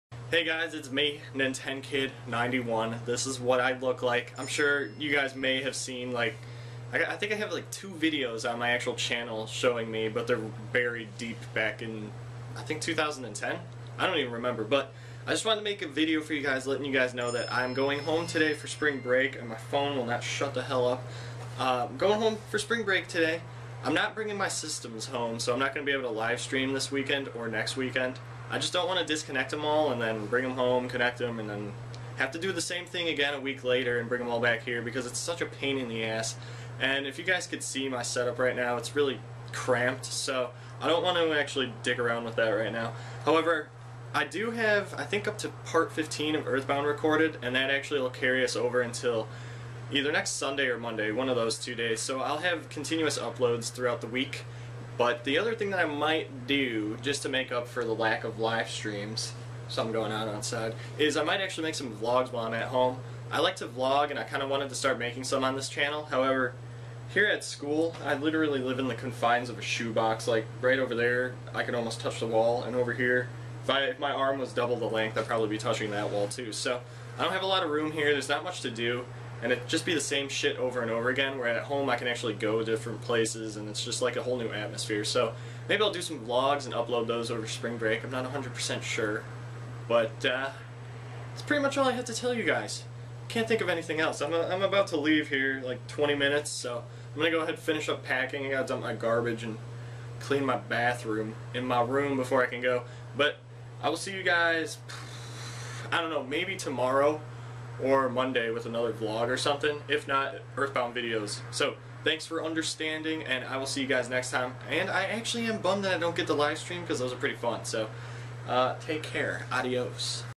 Spring Break Vlog (3/14/2014)
Okay so first of all this is me, nothing special.  Second of all this was supposed to go up on Friday but I kinda forgot to upload it when I got home.  Anyways I'll be making more of these over Spring Break as well as in the future on this channel.